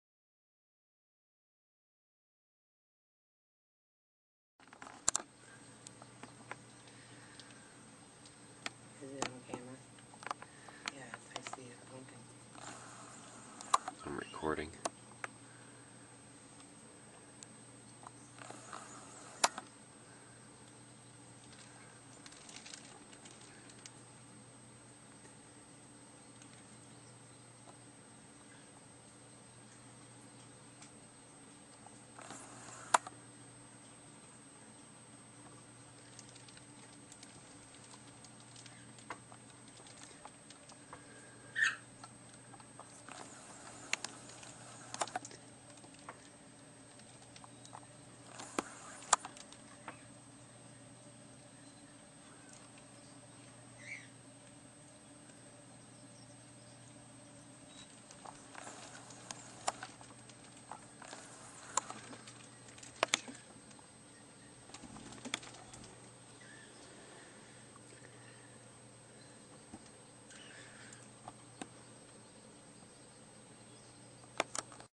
Fisher on March of 2010...suet thief.
This is 2nd largest of several fishers living in the Sentinel Mountain area.  Very similar to a wolverine in body and demeanor.